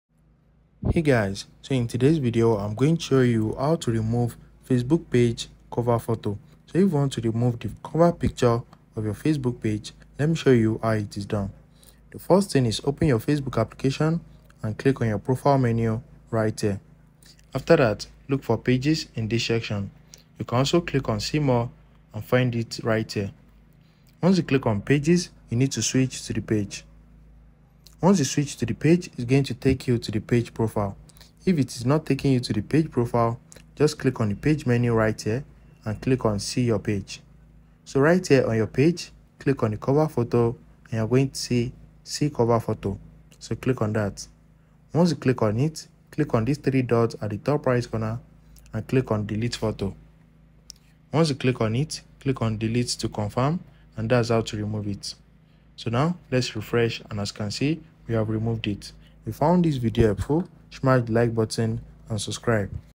How to Remove Facebook Page Cover Photo - Quick and Easy Guide!
In this video, we’ll guide you through the process of removing the cover photo from your Facebook Page. Whether you want to update your page’s design or remove an old cover photo, our step-by-step tutorial will help you do it effortlessly. Keep your Facebook Page fresh and relevant with ease.